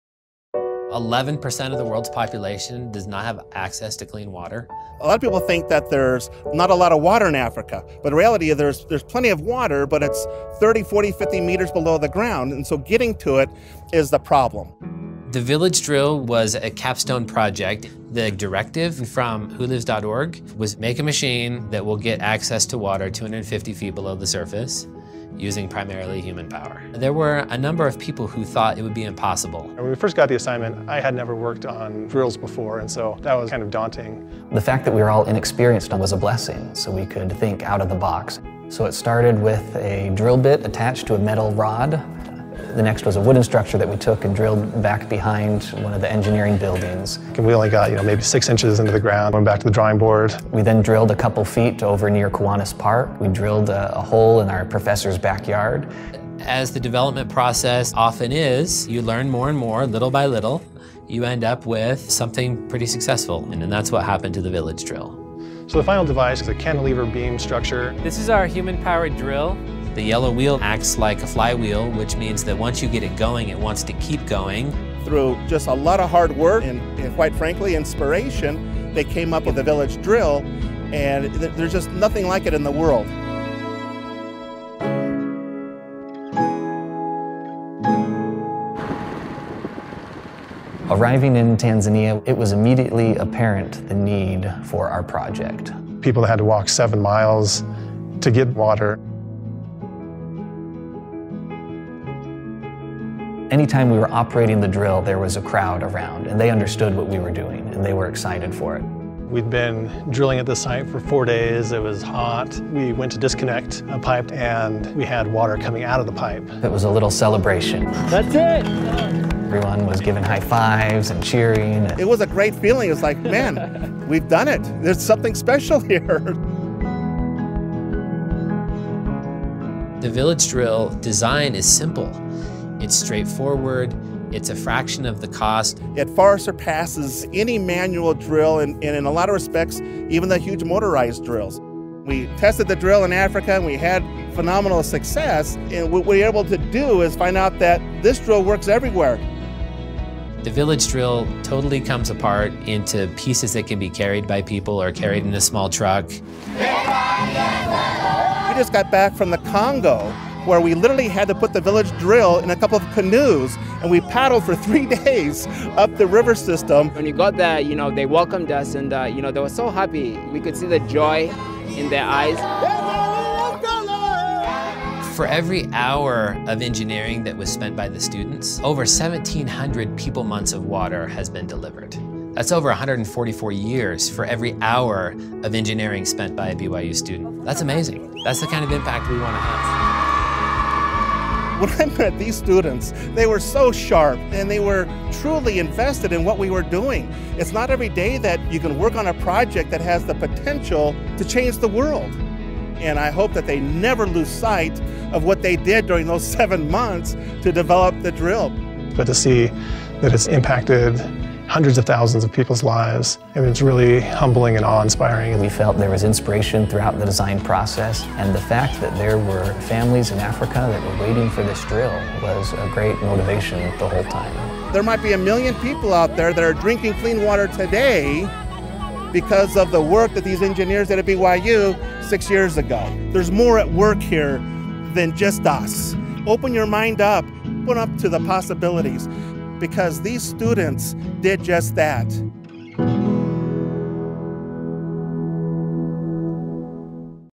The Creation of the Village Drill with BYU Capstone Program (Watch all 4 mins)
In 2020 this incredible device has provided clean water to 6 million people in 36 countries. Watch the story of the creation of the Village Drill with Brigham Young University. Considered one of the top achievements from the past decade at BYU, the original drill from 2010 is still going strong!